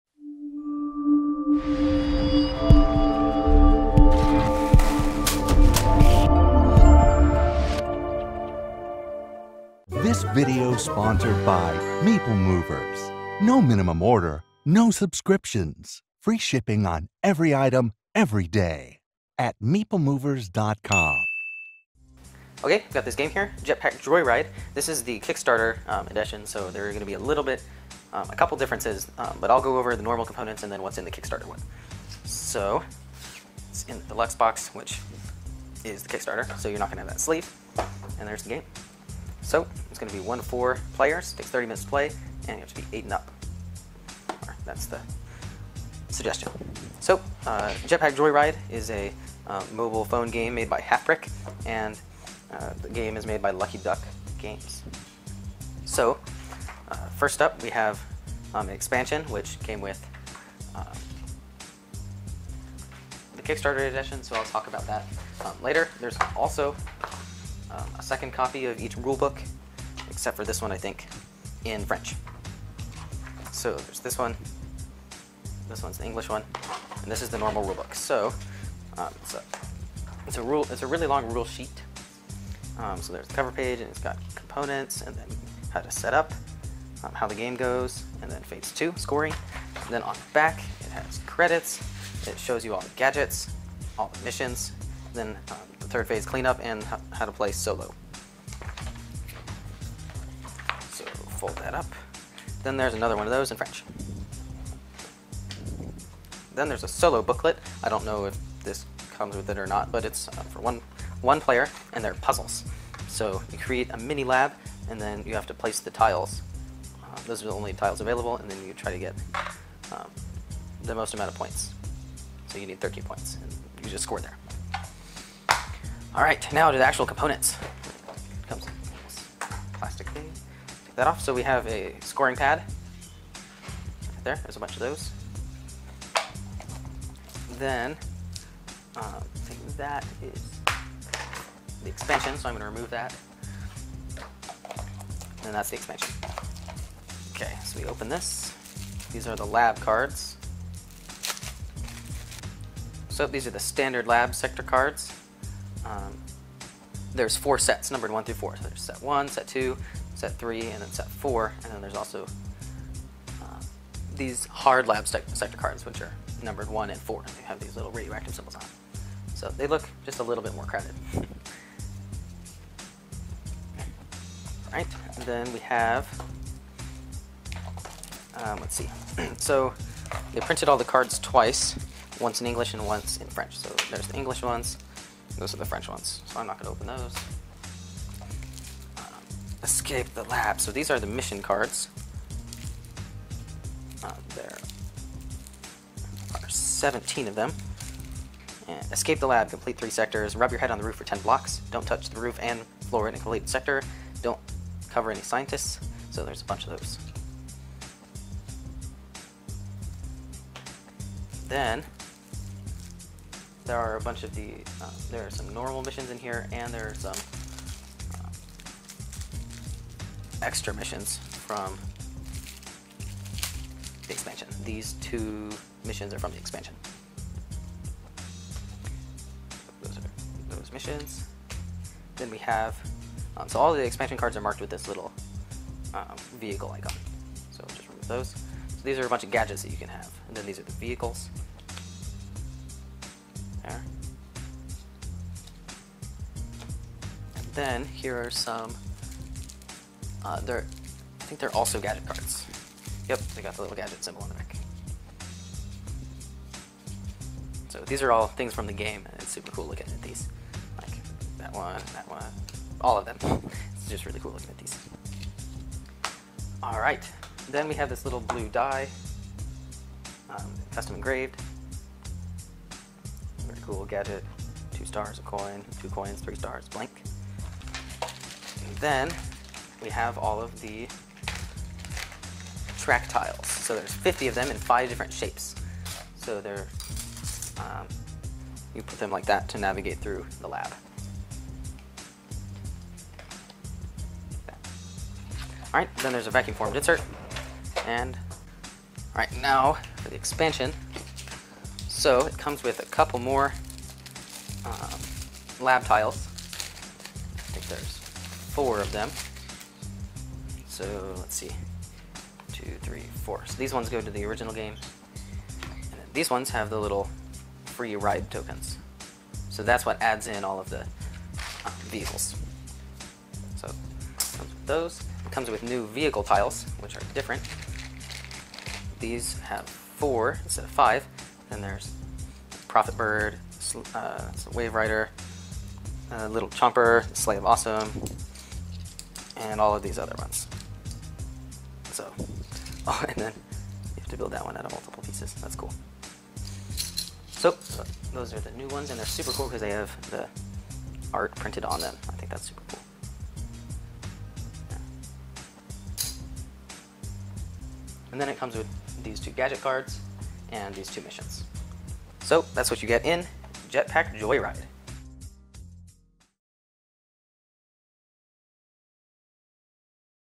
Unboxed - Jetpack Joyride
Brian opens up Jetpack Joyride by Michał Gołębiowski. It is based off the mobile app by the same name and published by Lucky Duck Games.